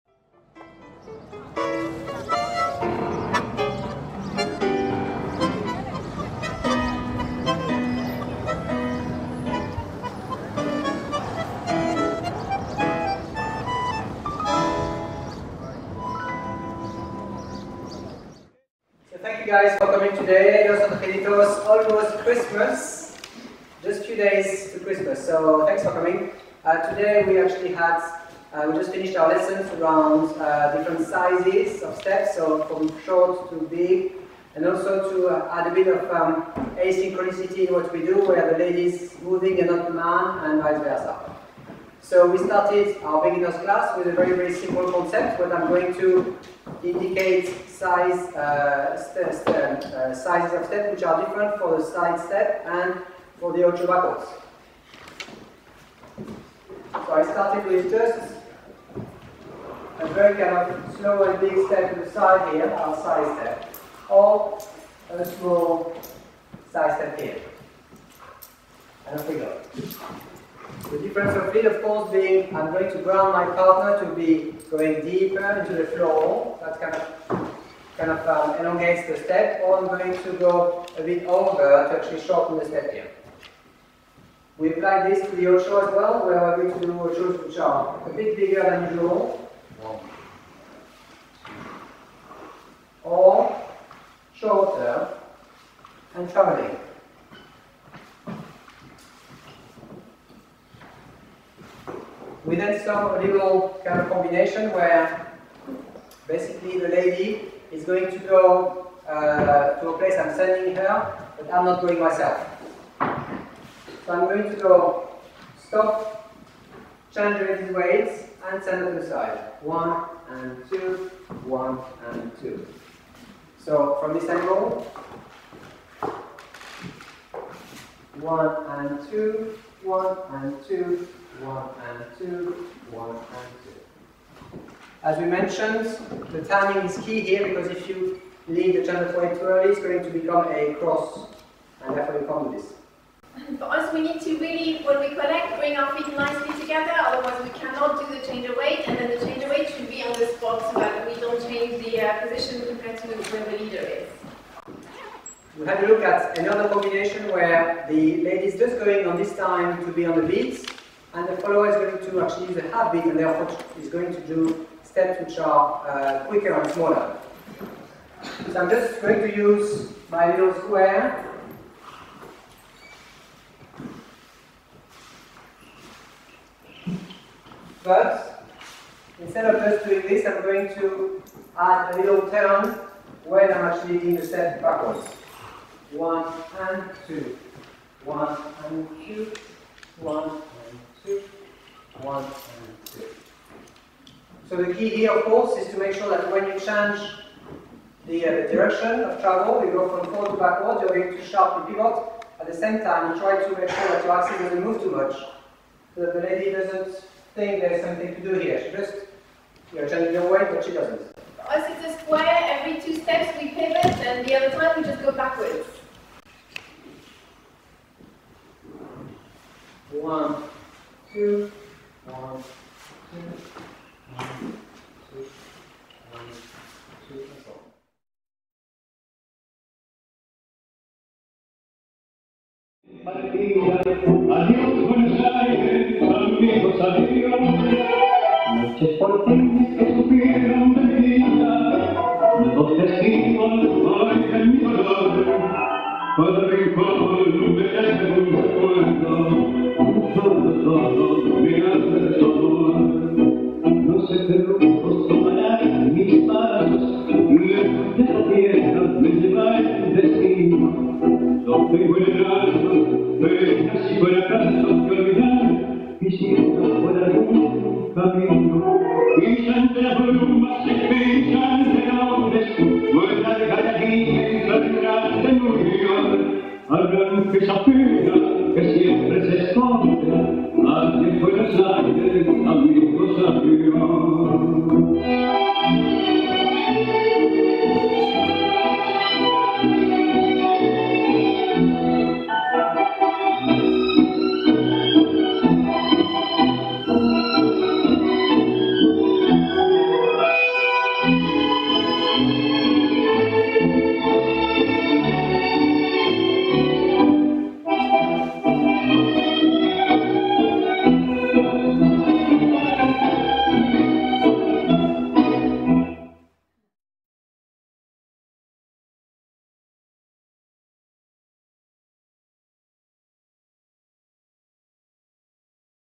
Tango size » Cadencia and square with different sized steps - 11.12.2016
» CLASS NOTES

In this first class of our short term, we had a look at how to vary the size of basic steps, before moving on to how the leader and the follower can both take different sized steps whilst maintaining a nice connection. We saw one of our favourite tango salon step, a little cadencia, which is cute, playful and musical as well as a little square inspired by Osvaldo and Coca.
This Argentine tango video and class notes are meant to help our students remember what they’ve done in class.

» TANGUITO CLOTHING
Tanguito clothing is available at tanguito.eu. Nati wears and Bruno wears